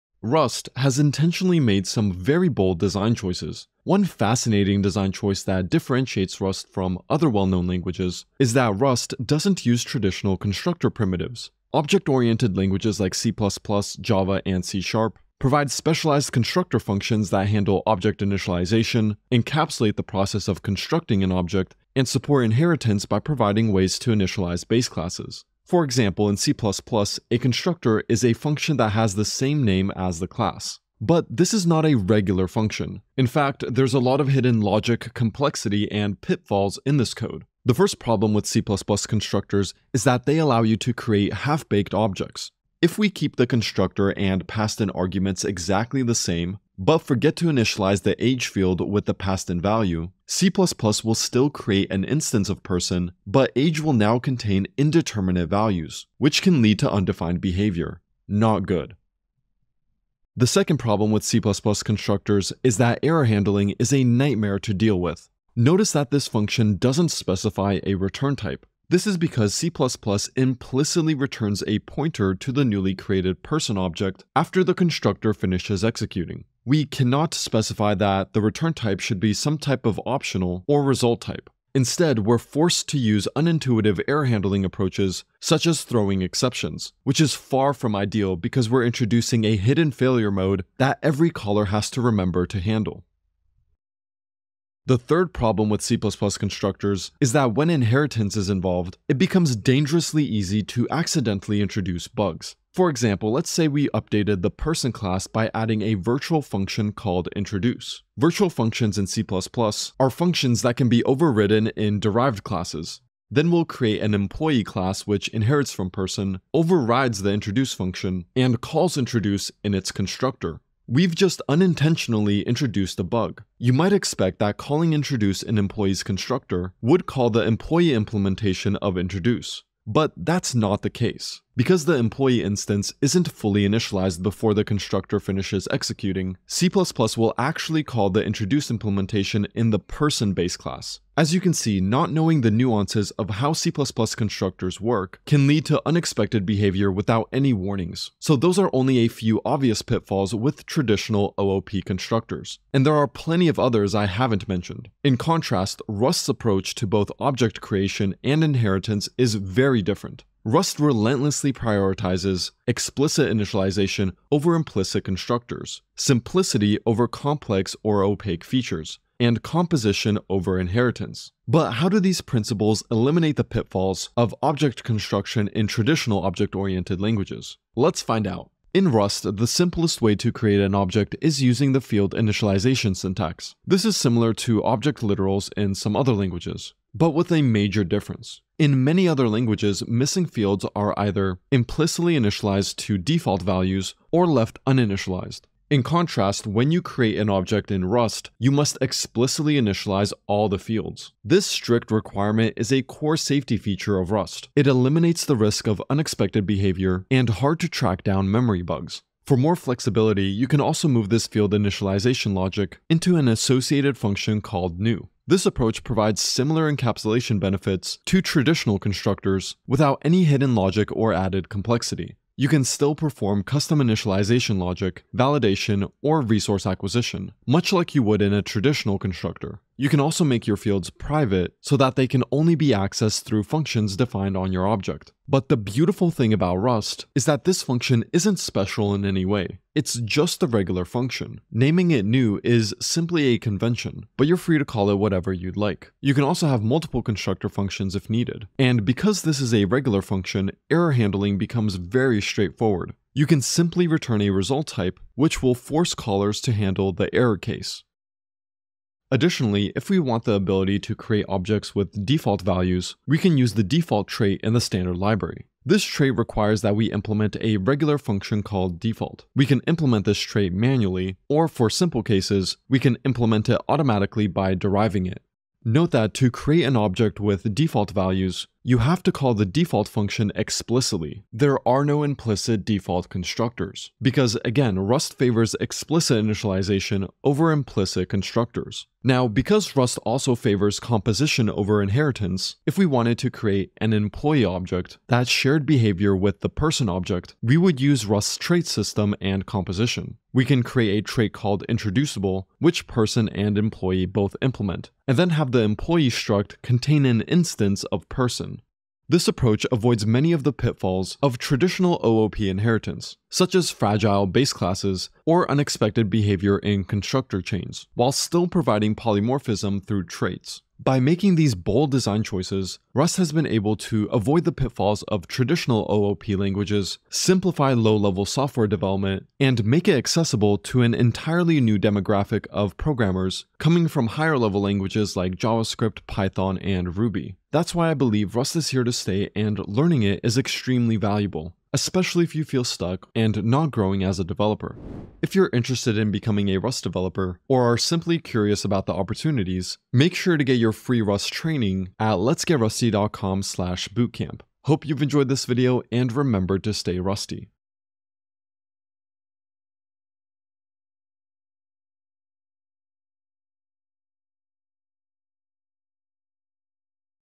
The genius of Rust constructors
· Dive into the bold design choices of Rust! In this video, we explore how Rust sets itself apart from traditional object-oriented languages like C++, Java, and C# by eliminating conventional constructors. Discover how this unique approach impacts object initialization, encapsulation, and inheritance. Whether you’re a seasoned developer or new to Rust, you’ll gain valuable insights into its innovative architecture.

Chapter:
0:00 Intro
0:25 C++ constructors
2:34 Rust's Approach